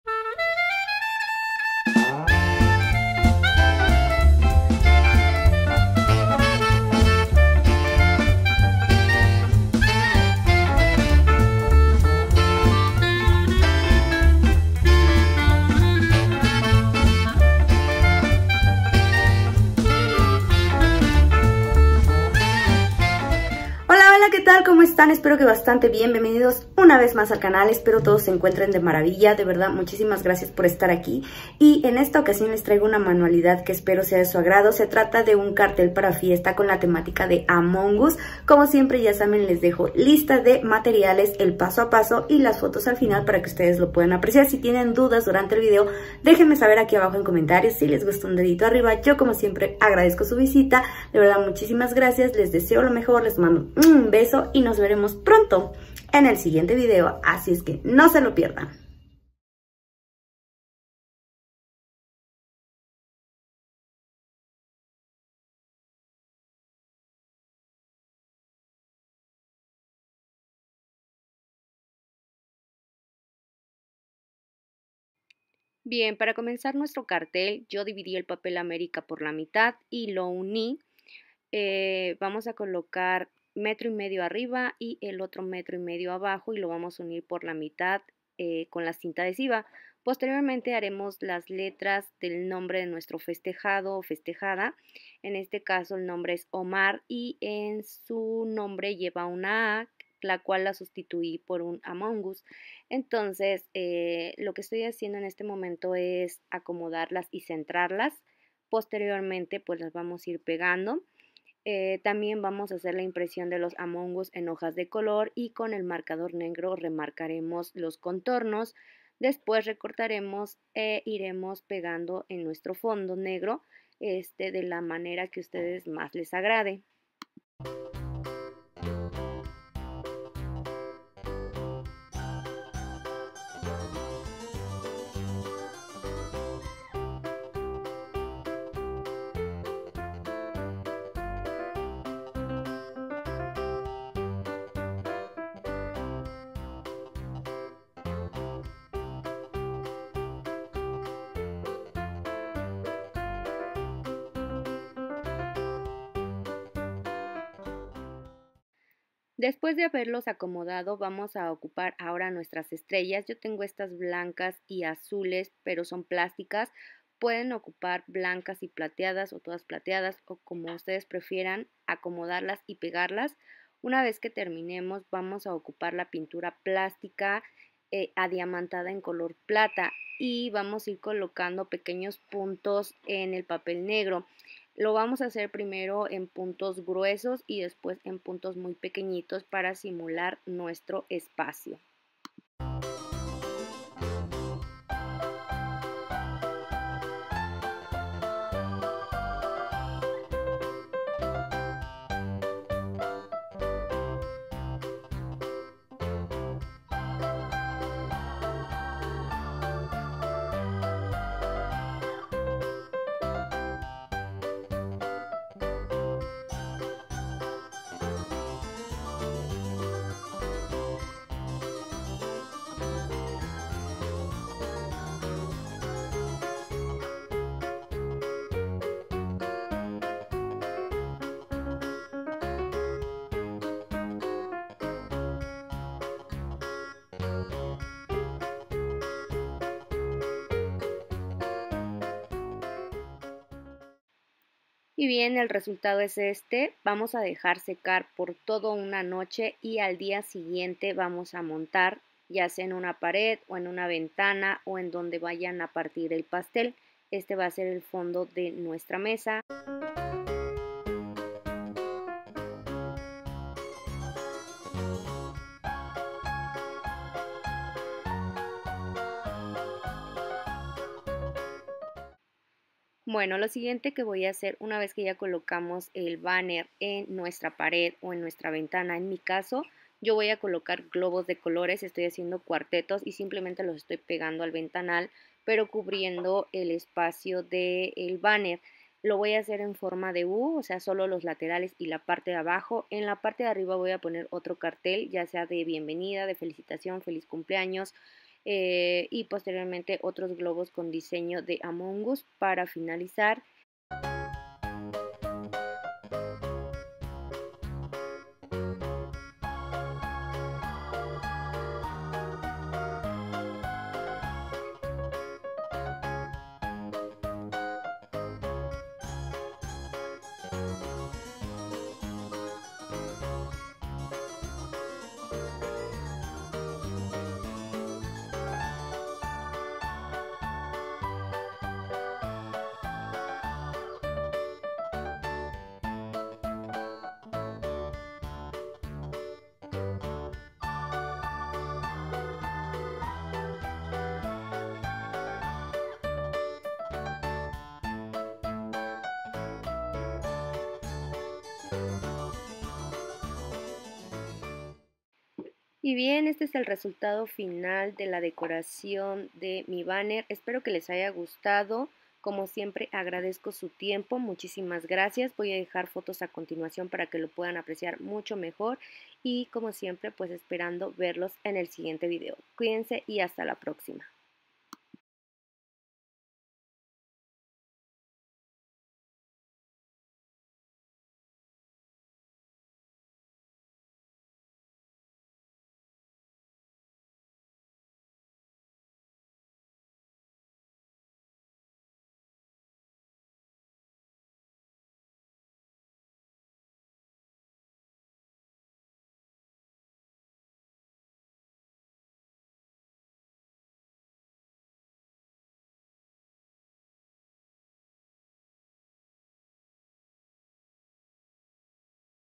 Cartel Para Fiesta - Among Us
Hola! hoy les muestro como realizar un cartel para fiesta con la temática de Among Us, esperando sea de su agrado, saludos y gracias por su visita...